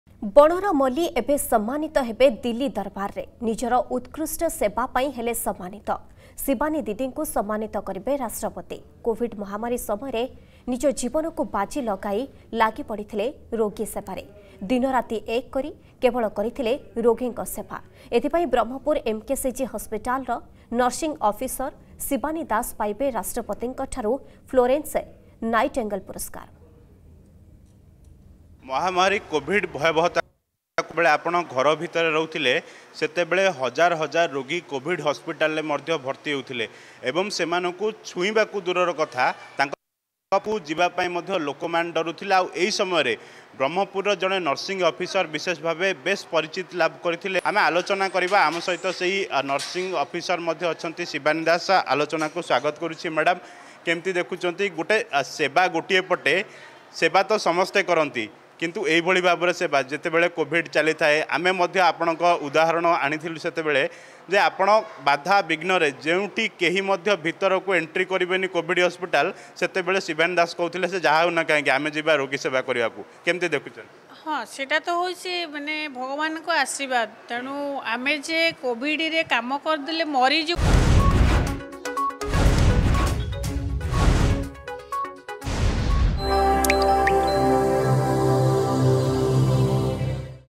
ବଣରମଲ୍ଲି ଏବେ ସମ୍ମାନିତ ହେବେ ଦିଲ୍ଲୀ ଦରବାରରେ
ବଣରମଲ୍ଲି ଏବେ ସମ୍ମାନିତ ହେବେ ଦିଲ୍ଲୀ ଦରବାରରେ। ନିଜର ଉତ୍କୃଷ୍ଠ ସେବା ପାଇଁ ହେଲେ ସମ୍ମାନିତ।  ଶିବାନୀ ଦିଦିଙ୍କୁ ସମ୍ମାନିତ କରିବେ ରାଷ୍ଟ୍ରପତି।